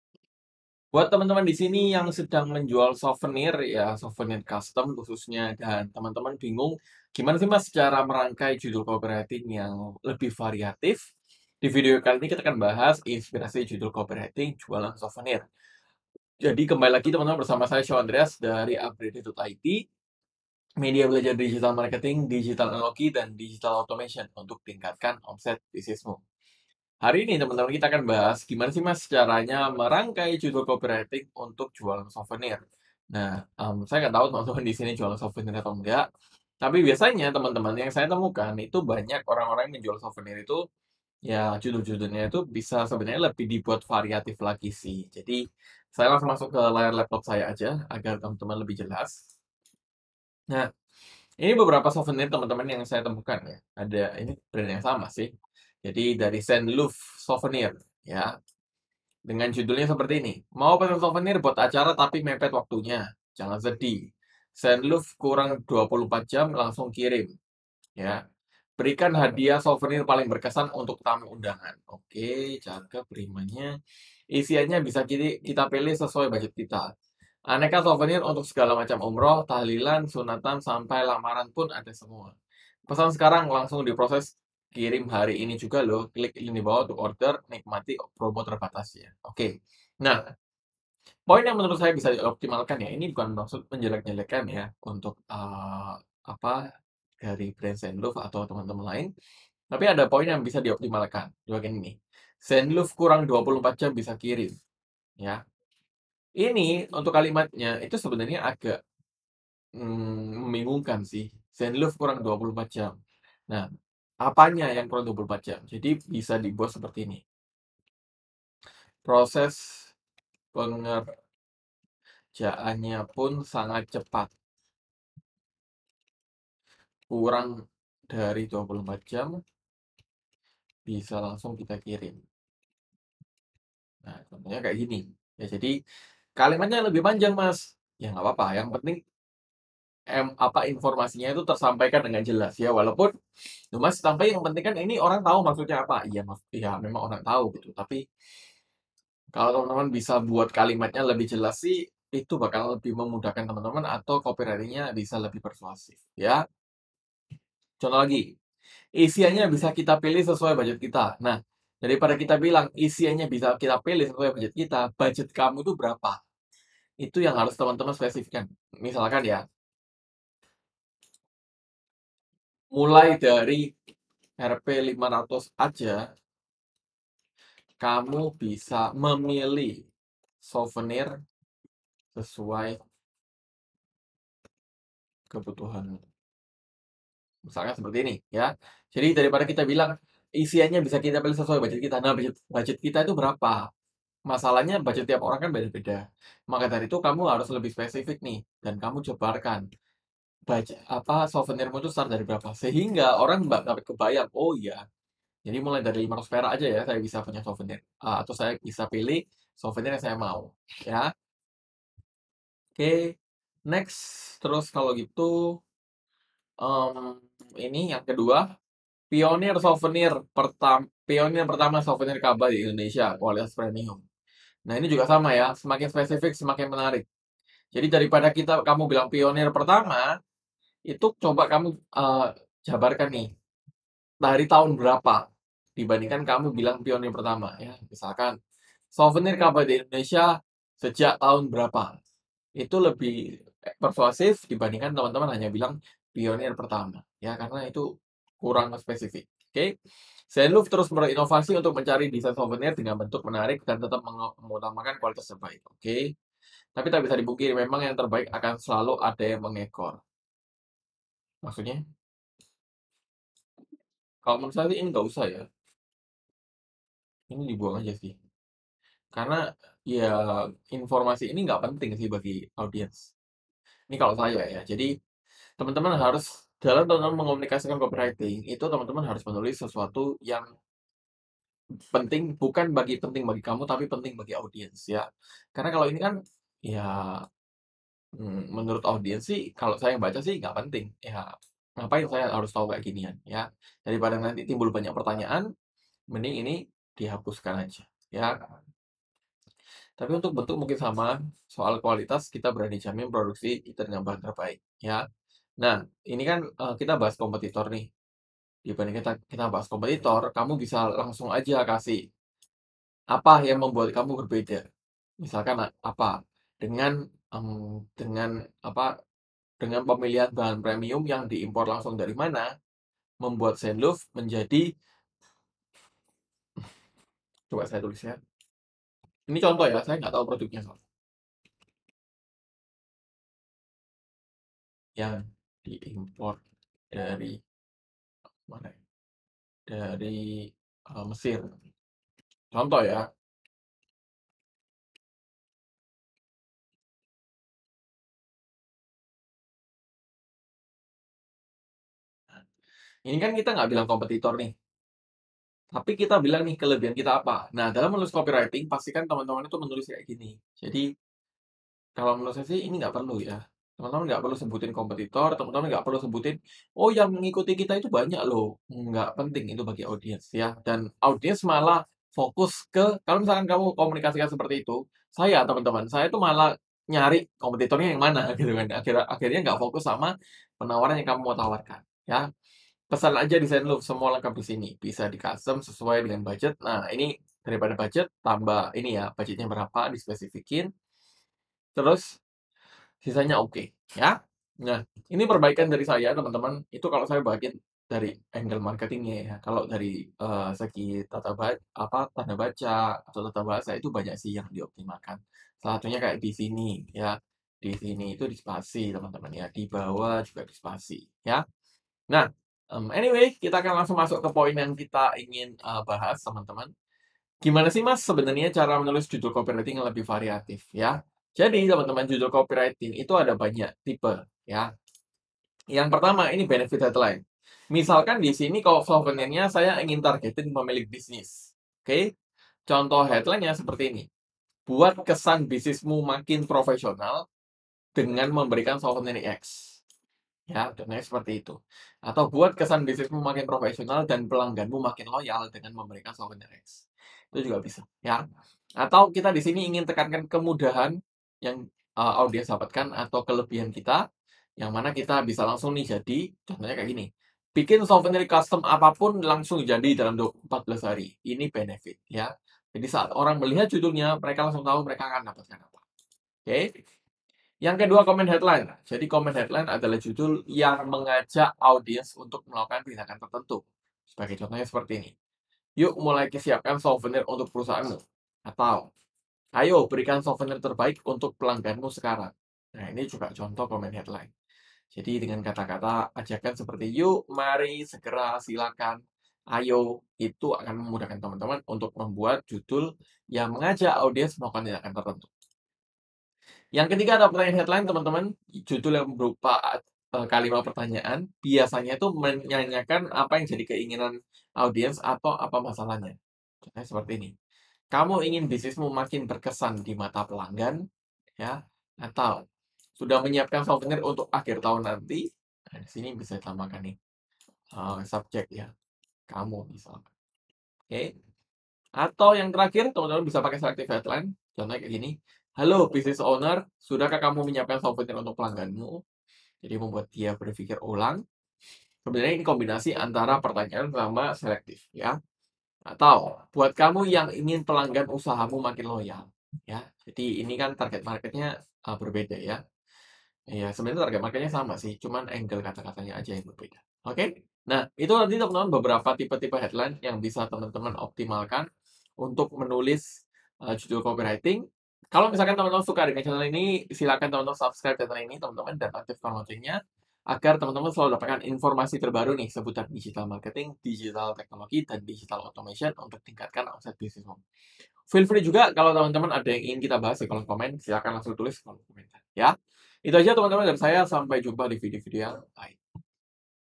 Inspirasi JUDUL COPYWRITING Jualan Souvenir
Kamu punya bisnis souvenir? Apa judul copywriting mu sudah dapat memikat calon pelangganmu? Yuk buat judul copywriting yang unik dan orderan souvenir mu akan lebih melejit!